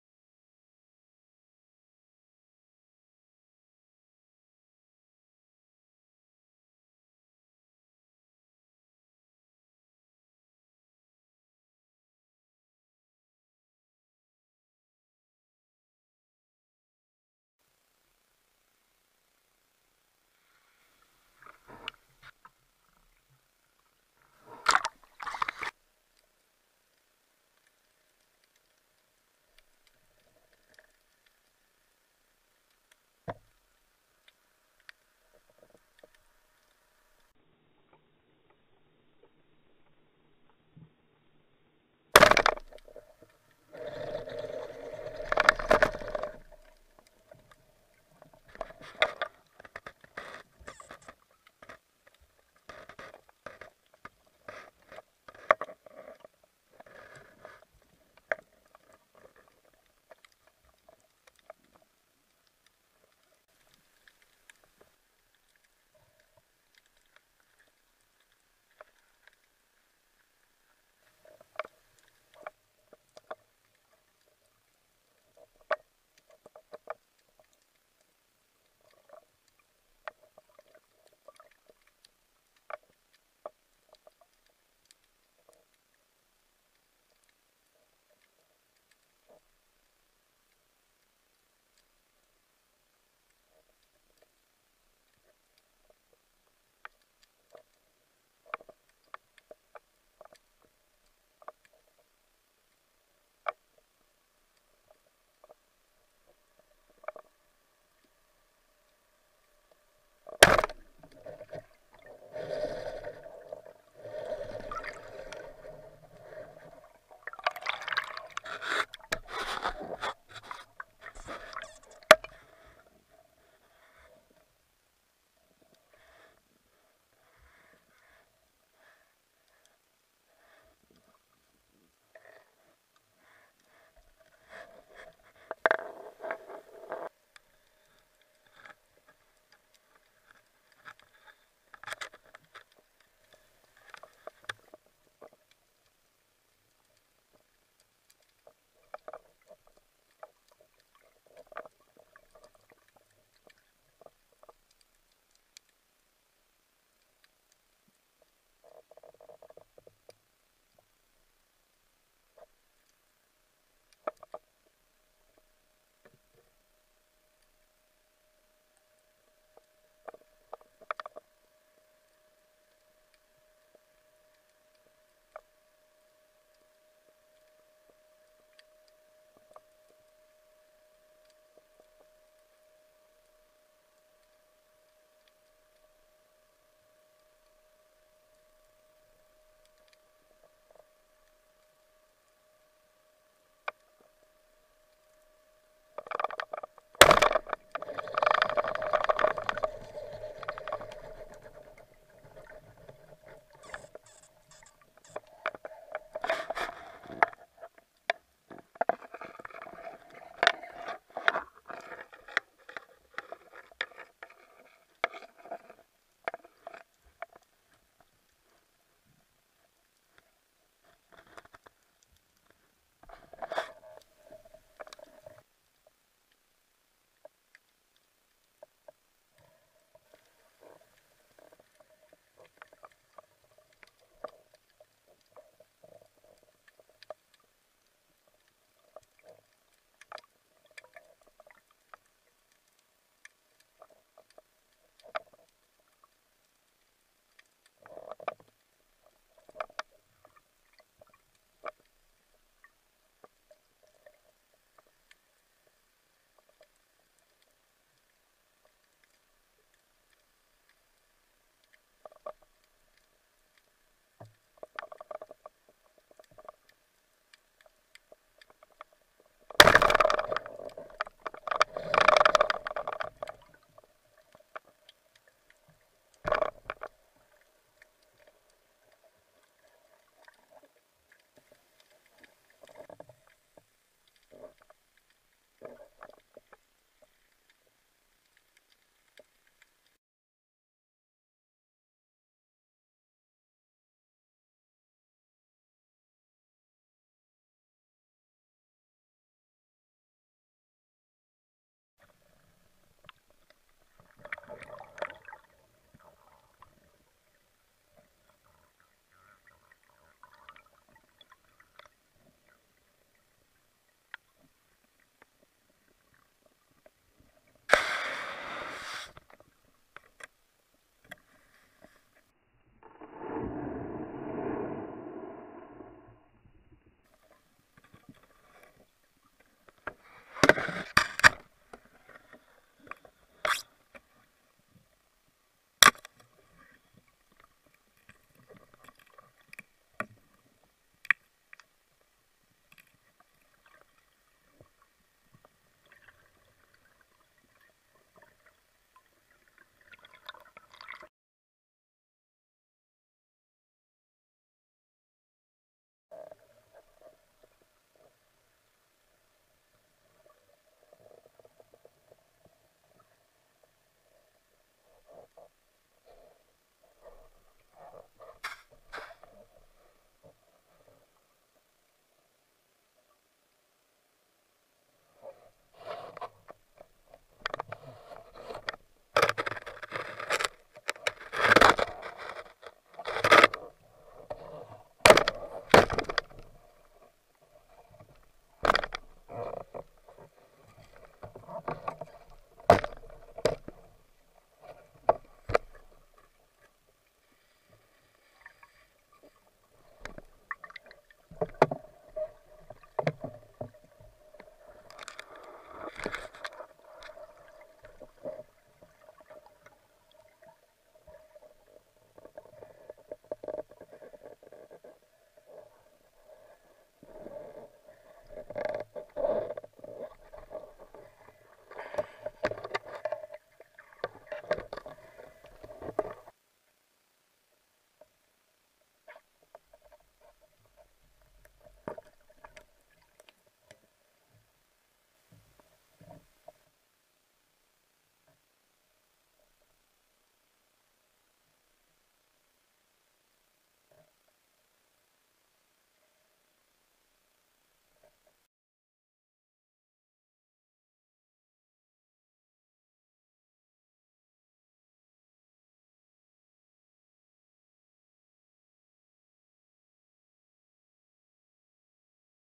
Pescasub: La danza dei cefali
Quattro catture in un bel branco di cefali dorati ed il solito grongo...